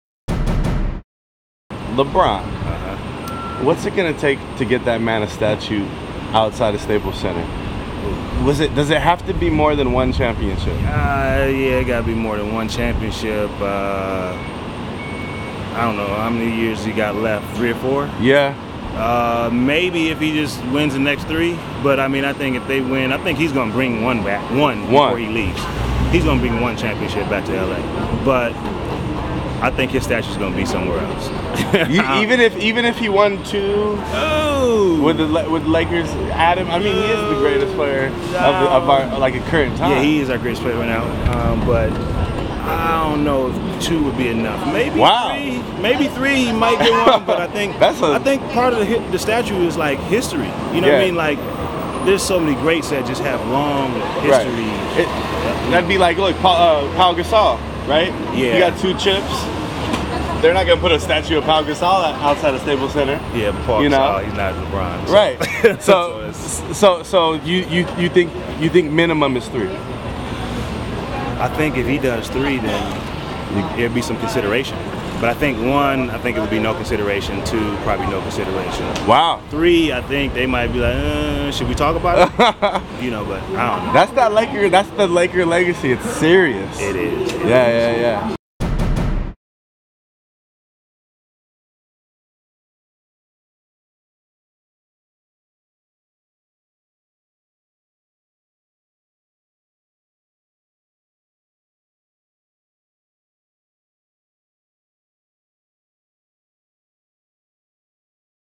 LeBron James Needs Three NBA Titles To Get L.A. Statue, Ex-Laker Says
Not one ... not two ... but THREE championships is what it'll take for LeBron James to get a statue among the legends outside Staples Center, so says ex-L.A. star Devean George.